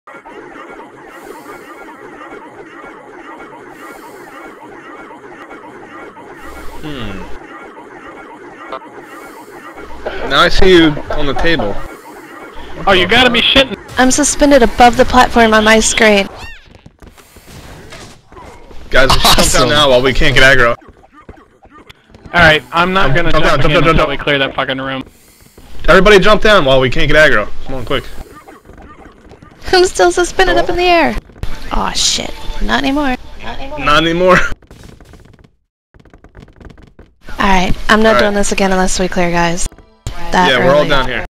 Steel Rake Glitch
Mass rubberbanding and failing during the jumps in Steel Rake.